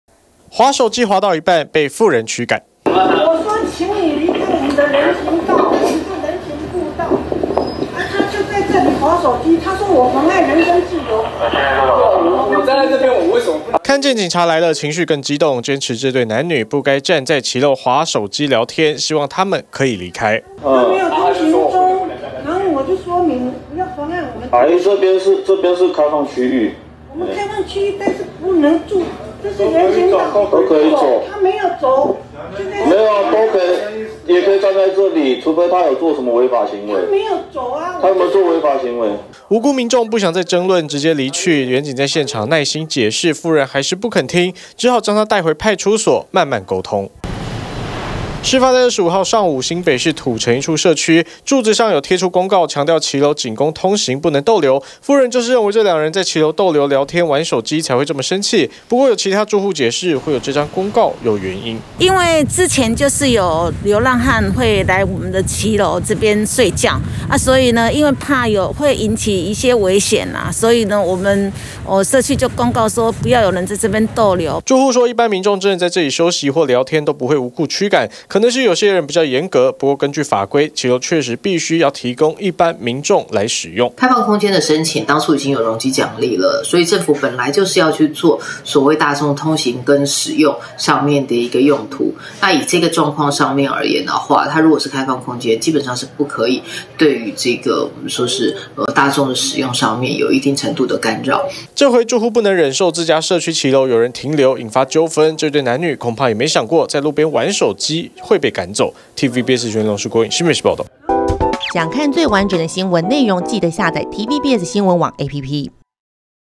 別站這! 路人逗留社區騎樓 婦驅趕爆糾紛｜TVBS新聞
新北市一名婦人，不滿有一對男女，在自家社區騎樓聊天、玩手機，上前驅趕爆發口角，警方到場後，婦人堅持騎樓只能提供通行，不能讓陌生人停留，儘管第一時間解釋，騎樓是公共空間，但她還是不肯聽，警方最後只好將她帶回派出所，好好溝通。